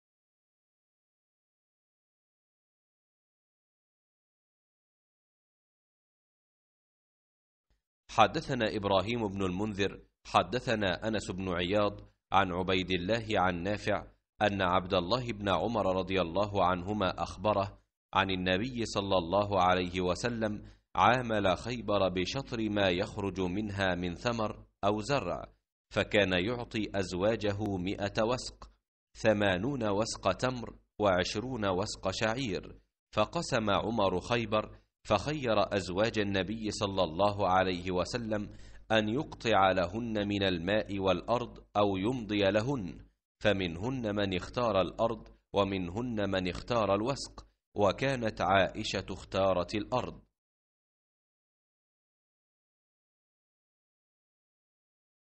صحيح البخارى - كتاب المزارعة - 2328 - باب المزارعة بالشطر ونحوه
حدثنا إبراهيم بن المنذر، حدثنا أنس بن عياض، عن عبيد الله، عن نافع، أن عبد الله بن عمر ـ رضى الله عنهما ـ أخبره أن النبي صلى الله عليه وسلم عامل خيبر بشطر ما يخرج منها من ثمر أو زرع، فكان يعطي أزواجه مائة وسق ثمانون وسق تمر وعشرون وسق شعير، فقسم عمر خيبر، فخير أزواج النبي صلى الله عليه وسلم أن يقطع لهن من الماء والأرض، أو يمضي لهن، فمنهن من اختار الأرض ومنهن من اختار الوسق، وكانت عائشة اختارت الأرض‏.‏